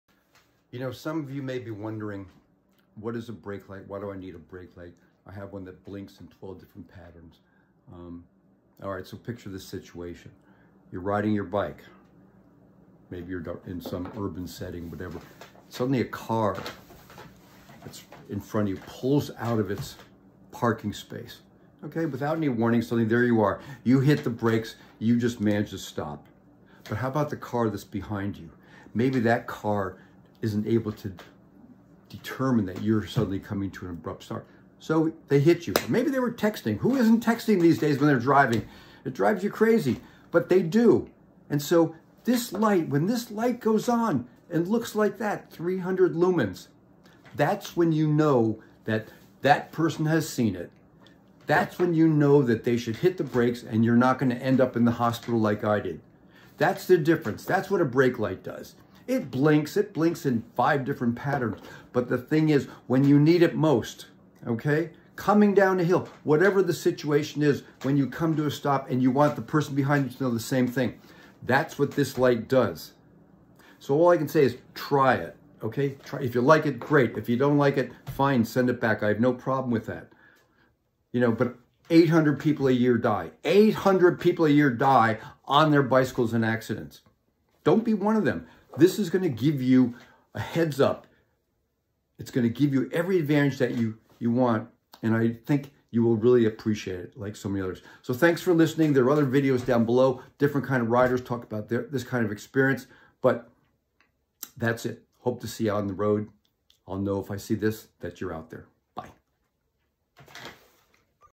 The history of the Tailgator Brake Light. Isn't it about time for a Bike Brake Light that works?
A bike brake light with 300 lumens of light, is a key bicycle safety product. I tell the story of the Tailgator Brake Light, how it is different from a rear bike tail light and why there is an urgent need for a high quality brake light. With over 800 deaths a year, safety for cyclists is critical. A bright, automatic, rechargeable  brake light will prevent accidents. Cars have had brake lights  for 80 years! Why not for bikes?? Attaches in seconds, no apps, bluetooth or wires. Here's my story and how we got here with the best bicycle brake light on the market today